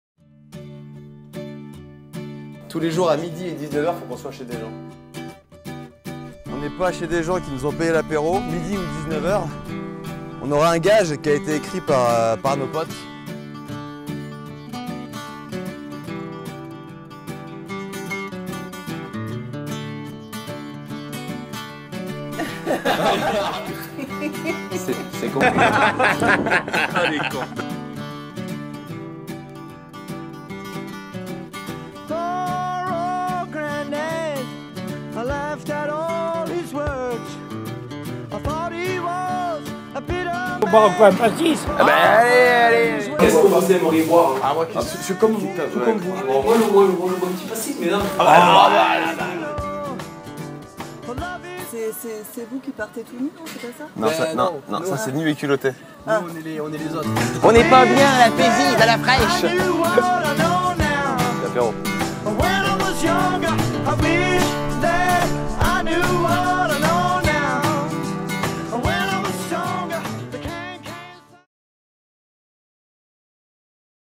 J'irai Prendre l'Apéro Chez Vous - TEASER
Quatre jours de marche et d'auto-stop à travers le Gard, une seule mission : Prendre l'apéro avec des gens, deux fois par jours.
Nous allons mettre notre périple en ligne prochainement, sous forme de mini-épisodes (Webisode, comme ils disent) de 3 - 4 minutes.

Le saucisson, le soleil, la convivialité, suivez-nous dans notre quête pour l'apéro.

Julien & Vincent